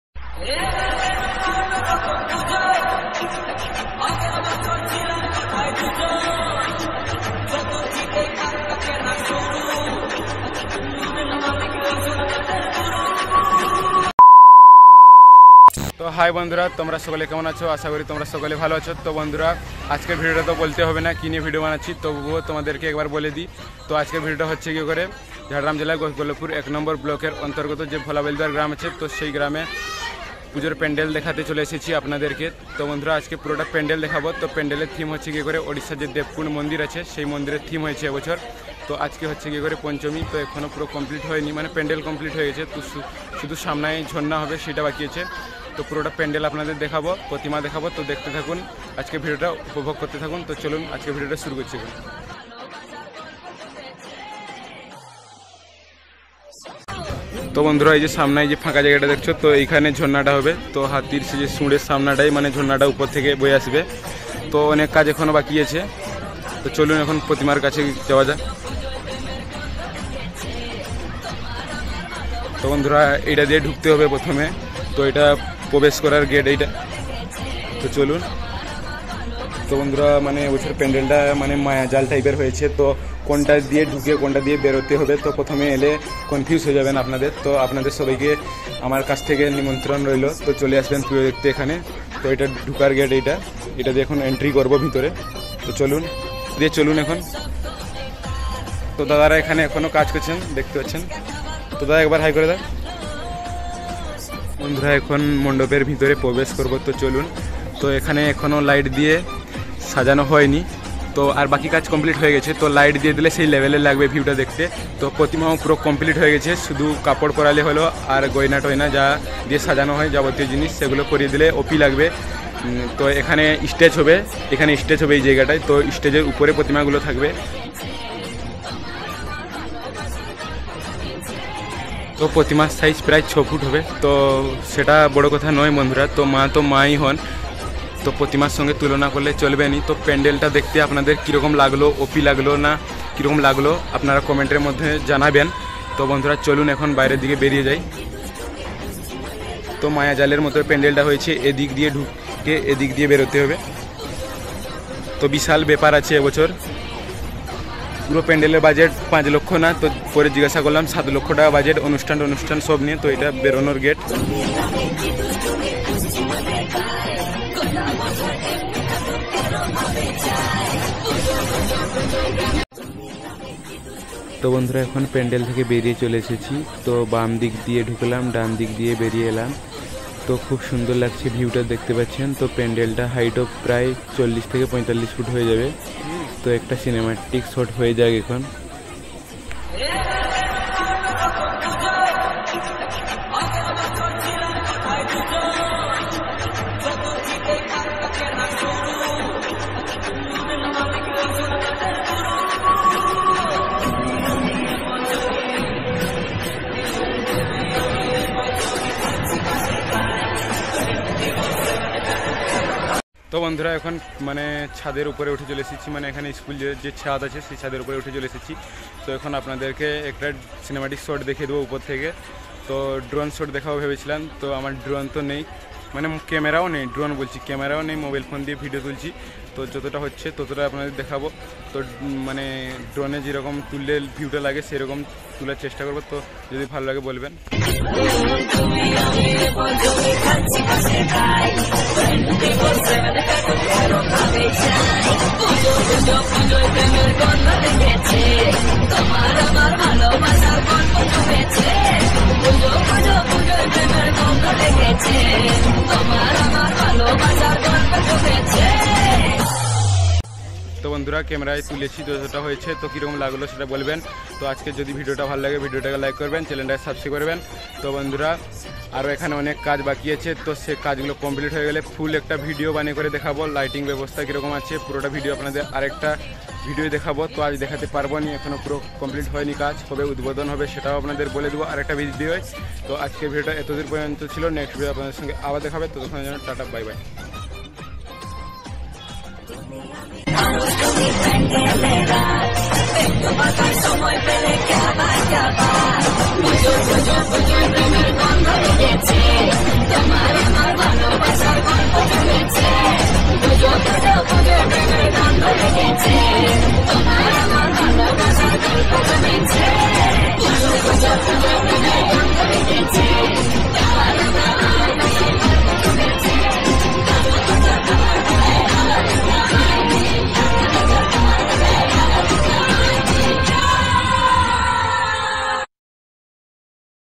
ঝাড়গ্ৰামের ভোলা বেলদুয়ারে এবারের দুর্গাপূজার থিম//উড়িষ্যার দেবকুন্ড মন্দির 🙏🙏
This year's Durga Puja theme at Bhola Belduar in Jhargram is Odisha's devkund Temple.